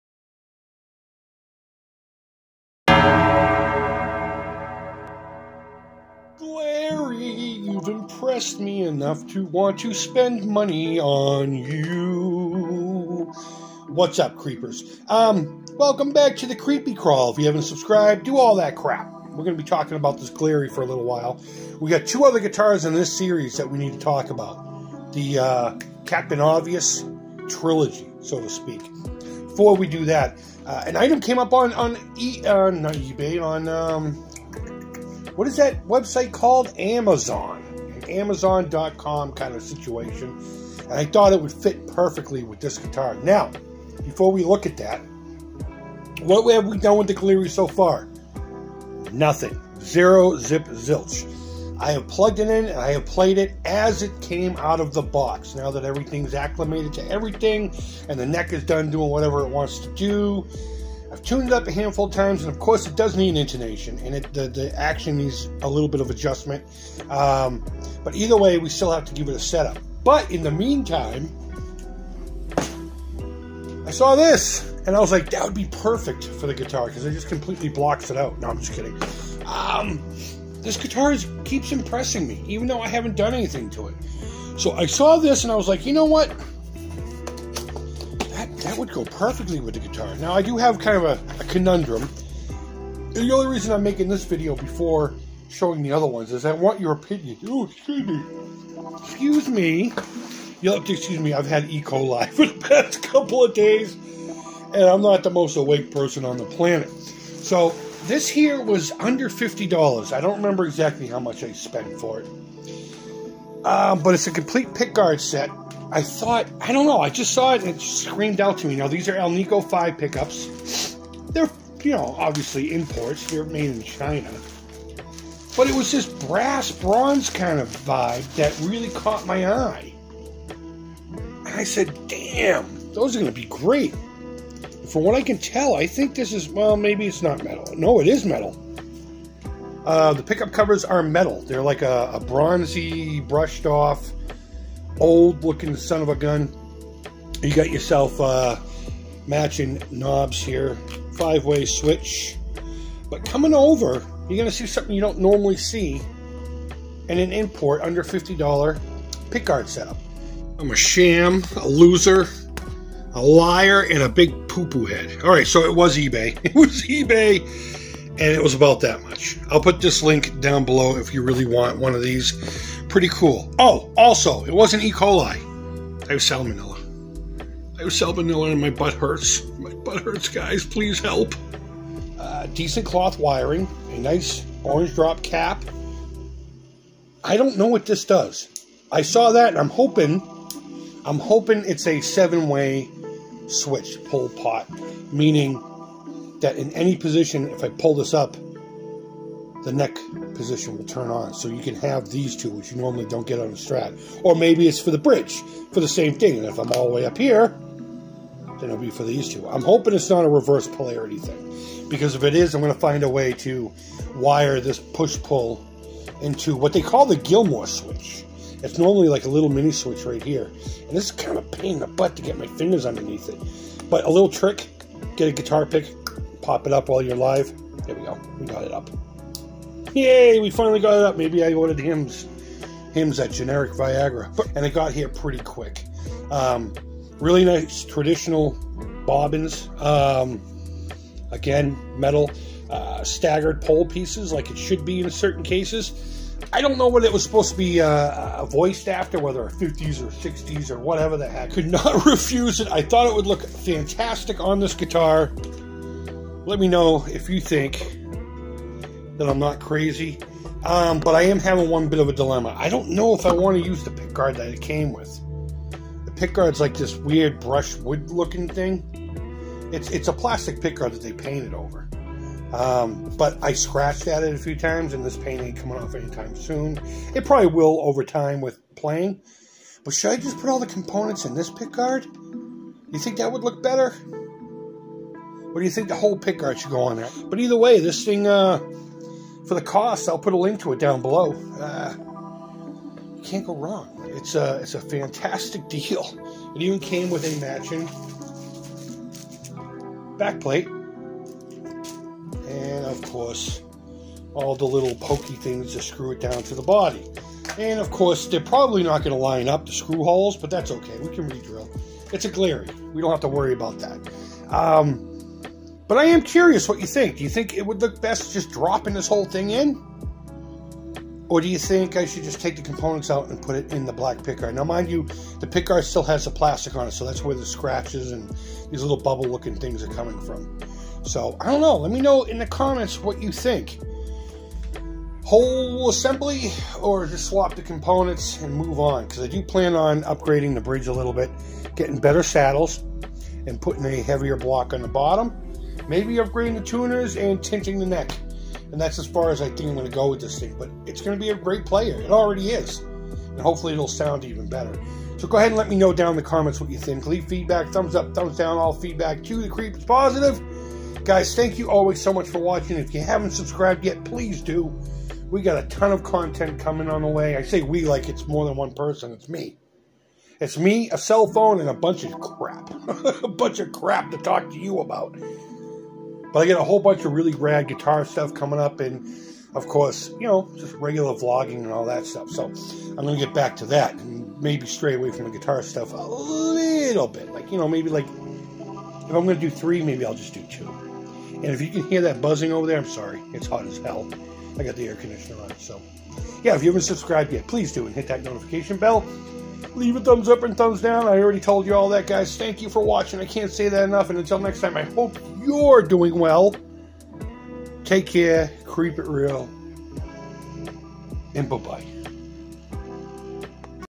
Sexy AlnicoV loaded pickguard for the Glarry GST
Welcome back,

So I have been playing the Glarry for a bit and I have to say, I really like it, A LOT! Came across this loaded pickguard on eBay and thought it would look (and hopefully sound) good on it. Question is, drop the entire pick guard on the guitar or just swap out the components to the black guard?

The Creepy Crawl
-Chris